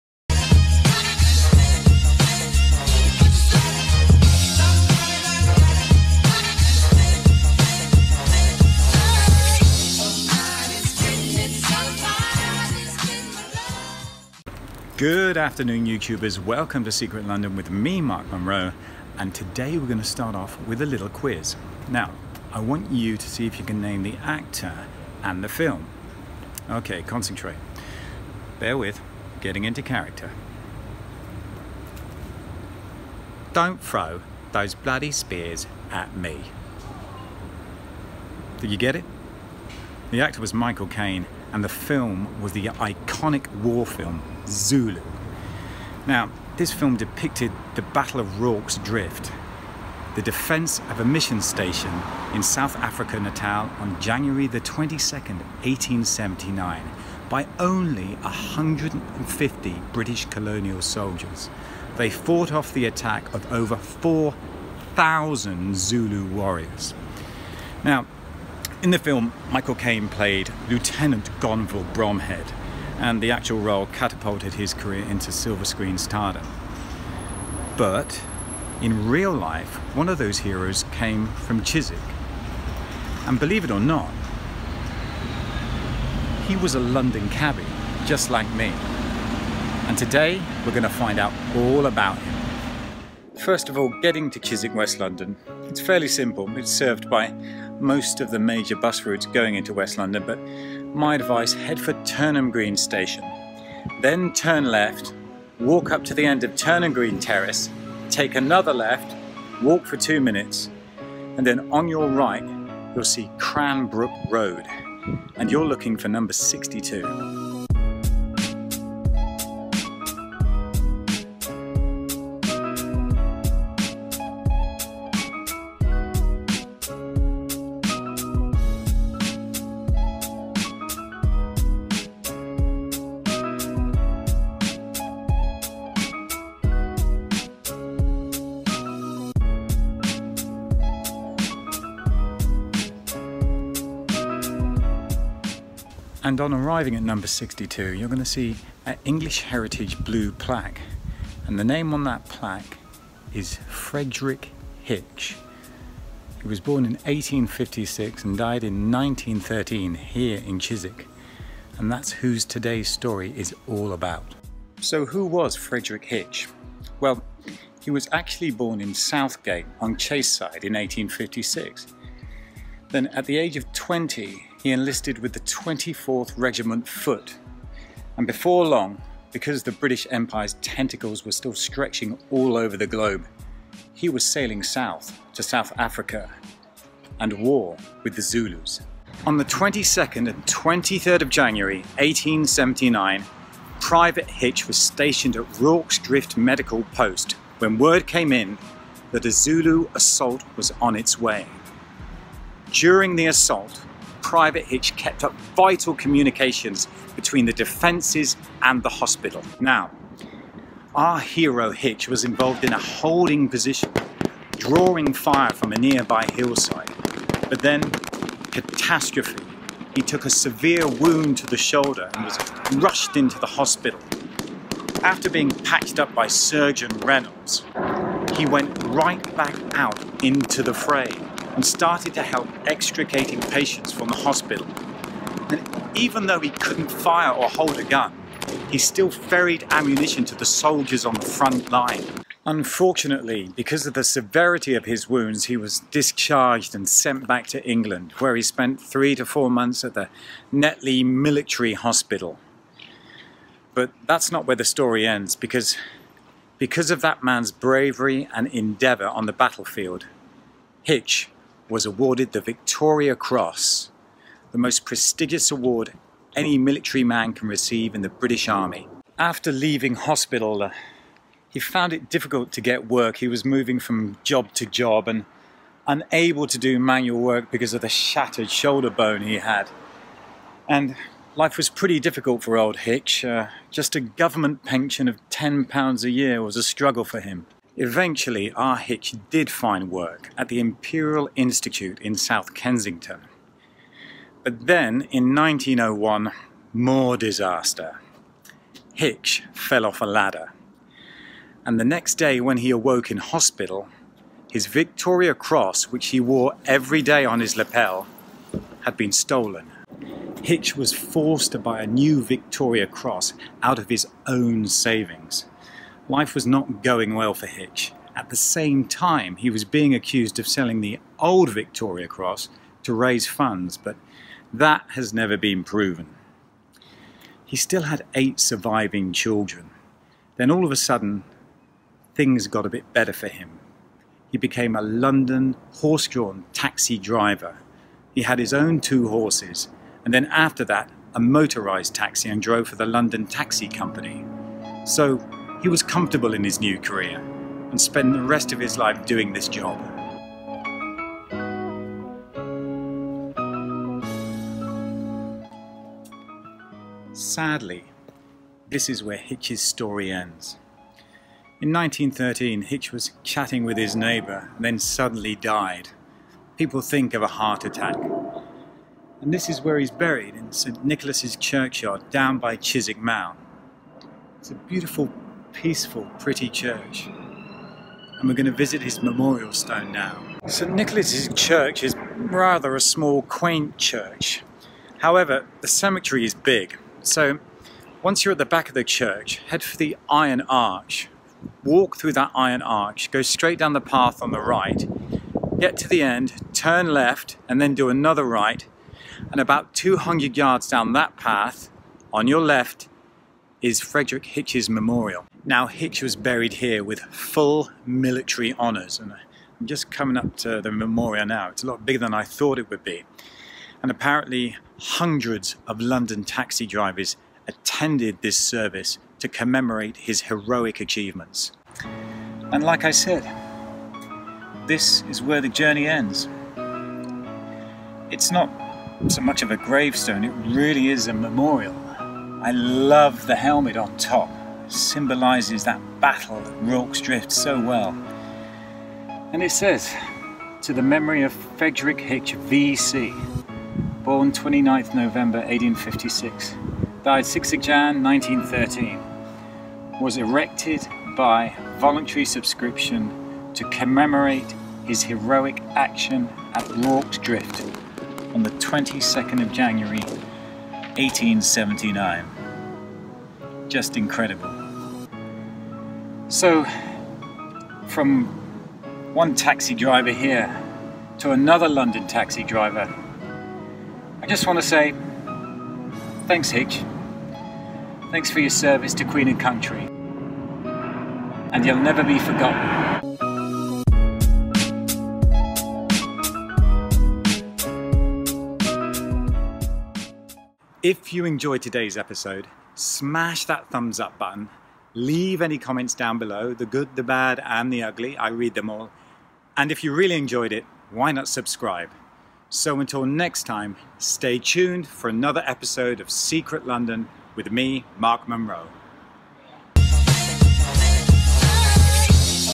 London taxi driver is hero at Battle of Rorke's Drift!
This amazing story tells of the heroism of a London taxi driver at the battle of Rorke's Drift in Natal South Africa.

Filmed on iPhone 7plus with Zhiyun smooth 4 gimble. National geographic tripod.